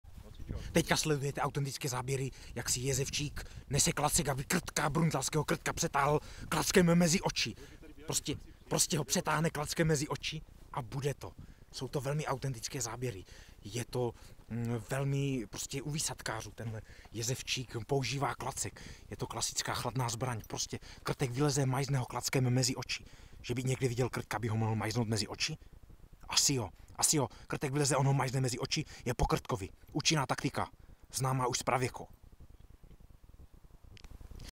NAPROSTÝ MASAKR!!! Lov krtků v Bruntále IV..
NAPROSTÁ BOMBA!!! Nová taktika!!!
Neuvěřitelné a autentické záběry z lovu na krtka z Bruntálu. Tito krtci jsou velci a NEUVĚŘITELNĚ agresivní...